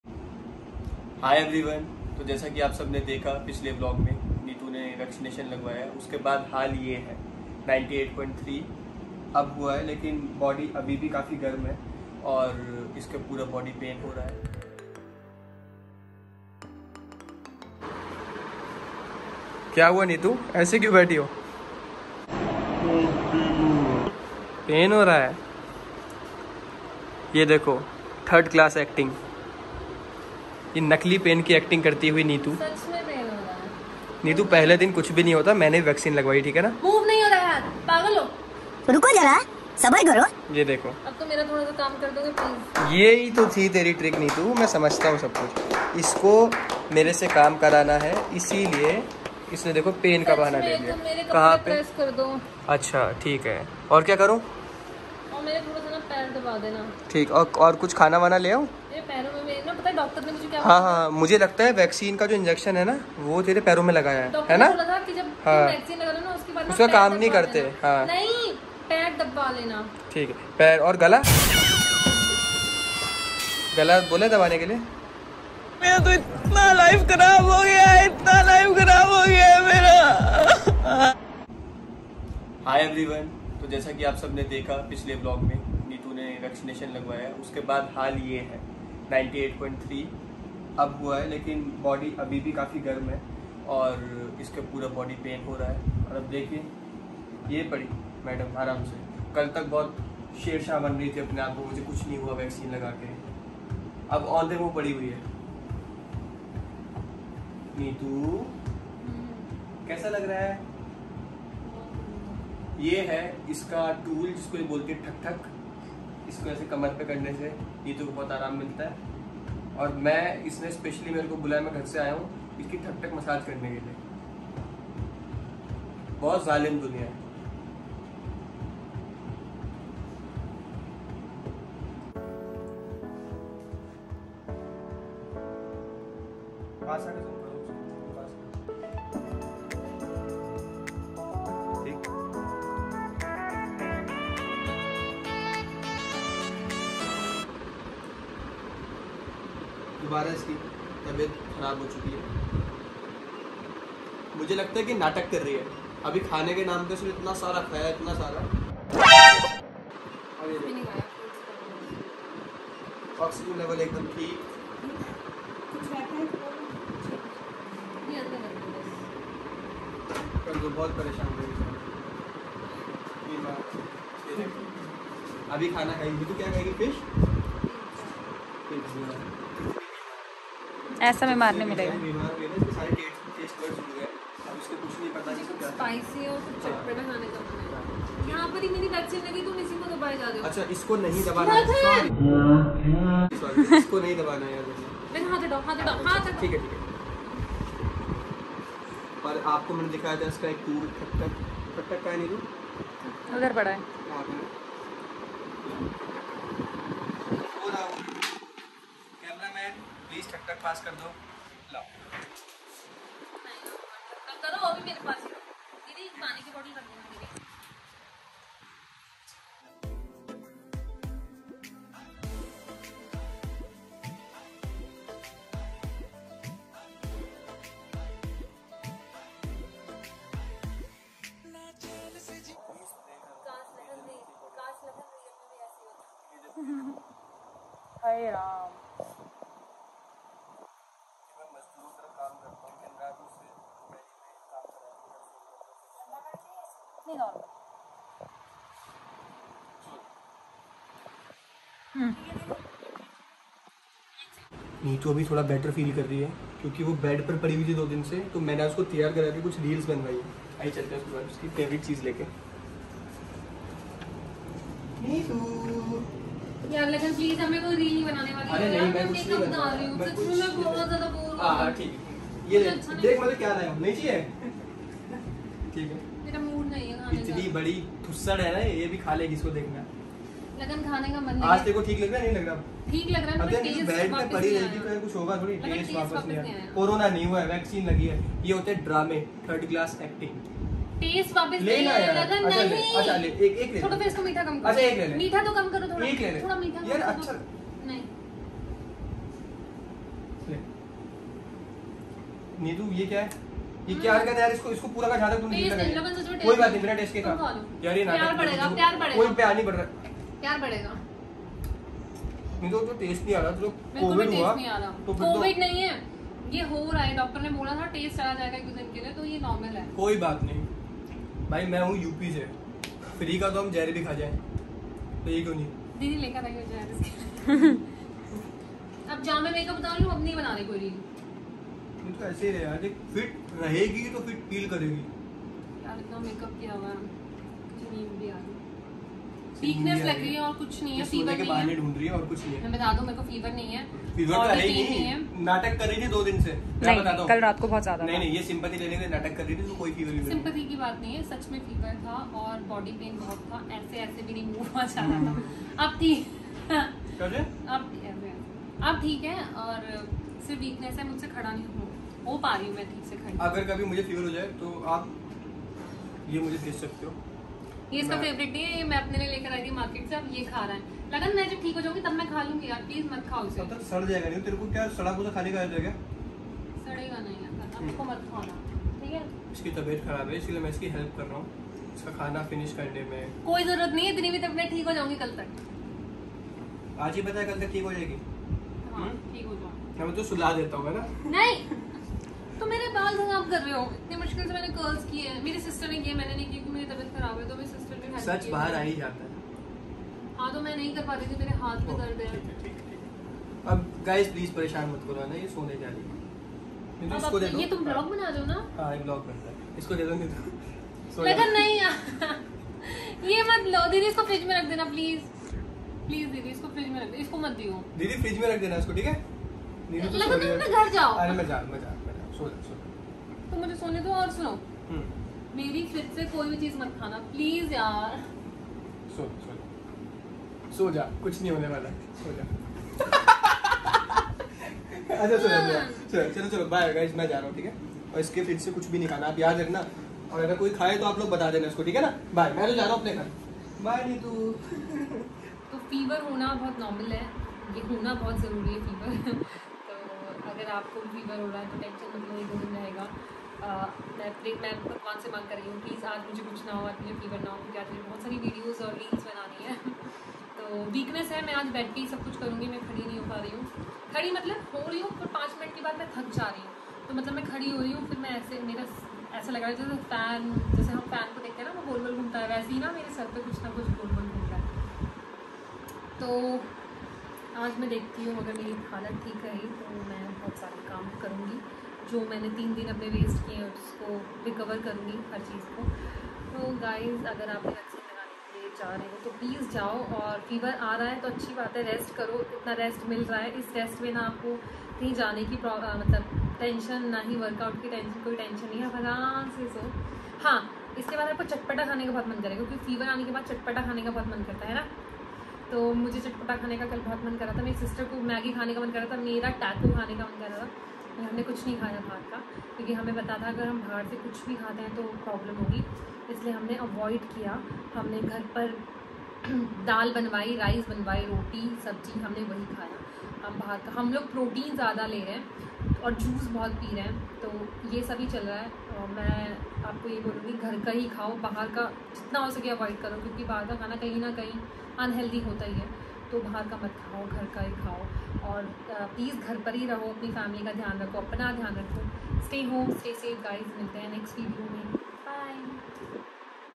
😕 NEETU AFTER DOSE 😭 | NEETU BISHT | LAKHAN ARJUN RAWAT | LAKHNEET VLOGS | COUPLE VLOGS
THIS IS WHAT HAPPENS AFTER WE TAKE 1st DOSE OS VACCINE,
BUT IT IS SAFE FOR SURE